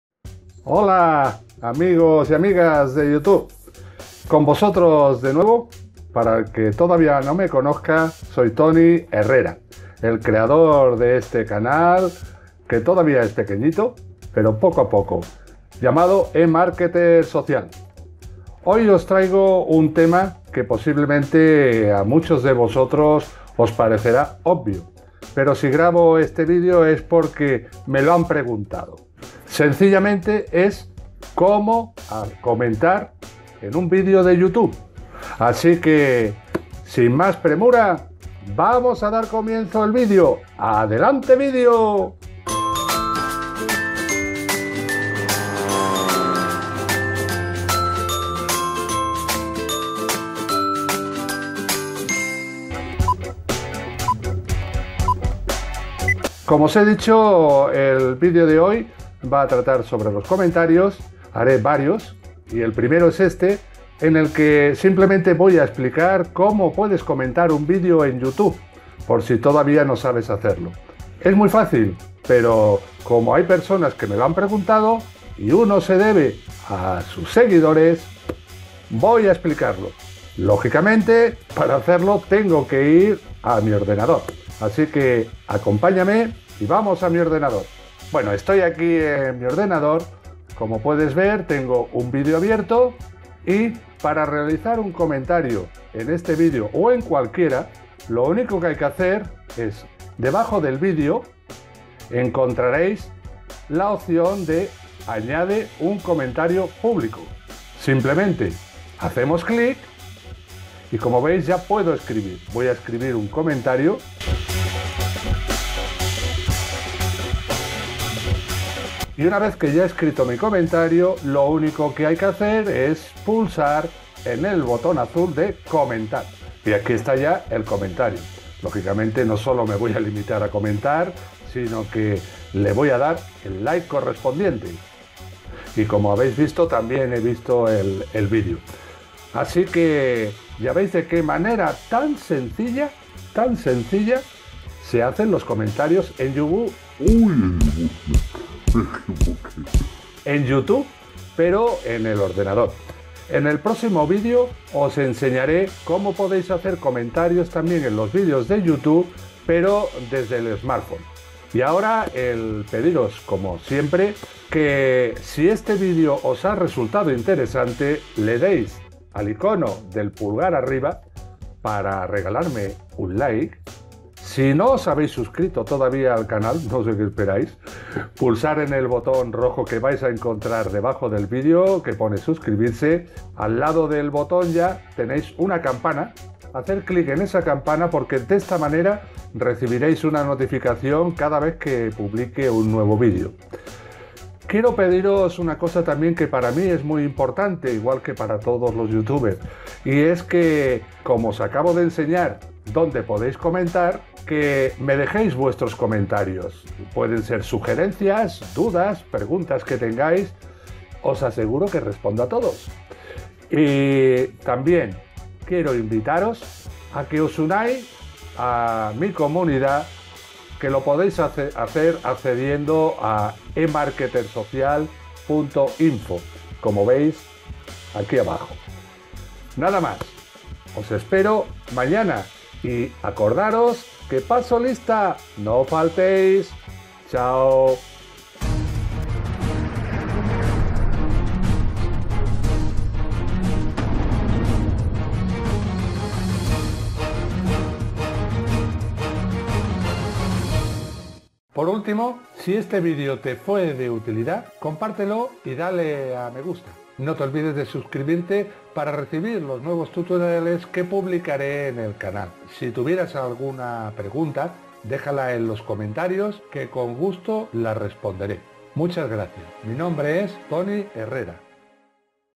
Cómo se comenta un vídeo de YouTube desde la computadora o PC 2018 | v45/365
Cómo se comenta un vídeo de YouTube desde la computadora o PC 2018. Para comentar un vídeo de YouTube desde la computadora primero debes disponer de la computadora o PC, como prefieras llamarlo, a continuación entrar en Youtube y visualizar un vídeo que quieras comentar. Por supuesto debes de tener acceso o conexión a internet ya que si no esto no será posible. Ji Ji Ji.

Aprende a comentar en YouTube de una forma fácil y divertida

Visto esto pues, vamos a ver cómo comentar en YouTube o, lo que es lo mismo,  como comentar en YouTube ya que es muy sencillo.

Si quieres comentar un vídeo de YouTube lo único que vas a tener que hacer es hacer un poco de scroll con el ratón y debajo del vídeo que acabas de ver verás una inscripción que dice "Añade un comentario público..." simplemente haces clic con el ratón ahí y ya puedes escribir tu comentario.

Bueno pues ya has visto lo sencillo que es el hacer un comentario en Youtube.

también, si tuvieras alguna pregunta, déjala en los comentarios que con gusto la responderé. Muchas gracias.

Te acompaña en tu camino hacia el Éxito
Toni Herrera